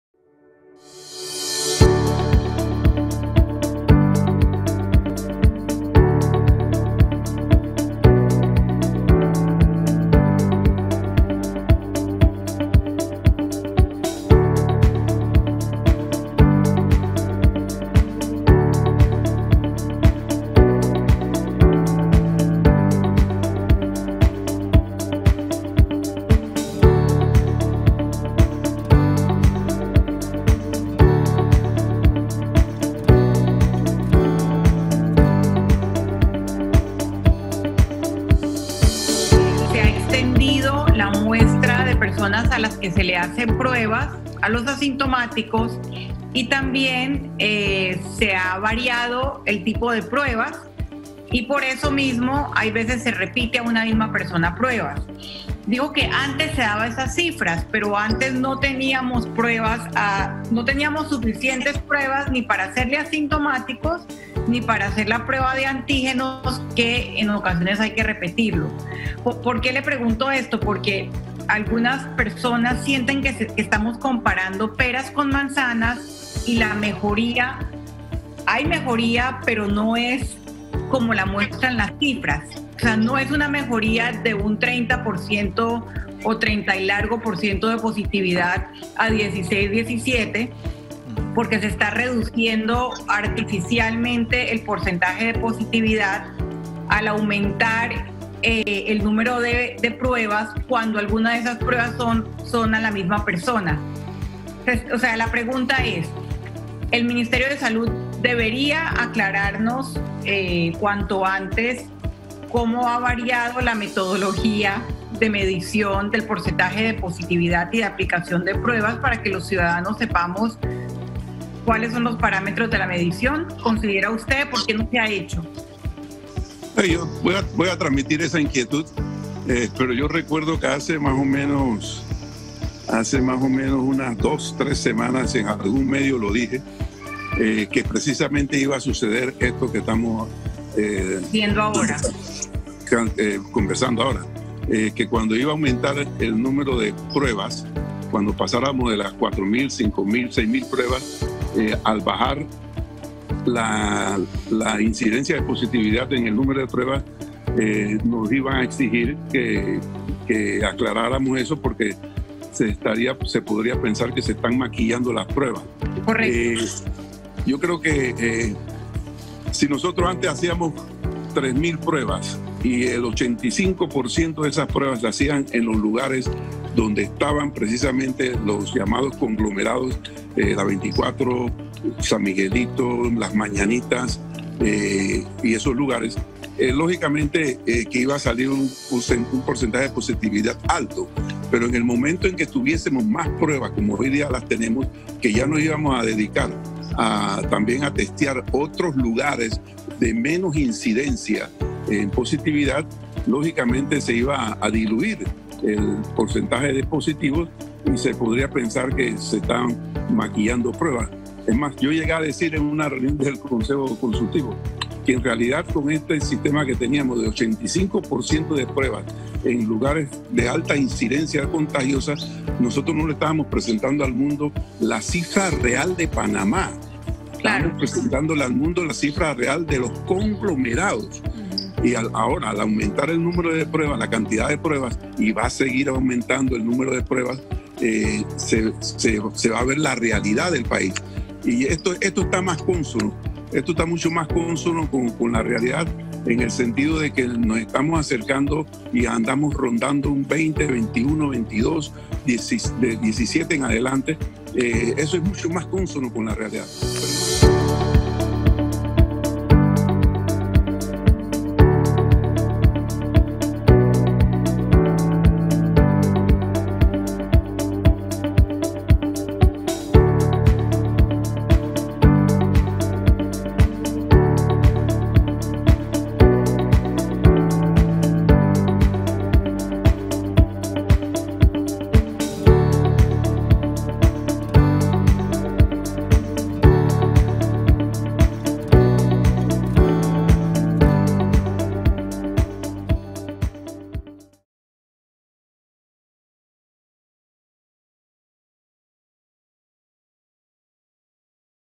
La estrategia del MINSA es maquillar cifras 2582020
Sánchez Cárdenas confirma lo denunciado por FRENADESO, la estrategia del MINSA es maquillar cifras.
Confesó que se fueron a otros sitios para reducir el porcentaje de positividad y los casos positivos.

Eso es falsear la realidad.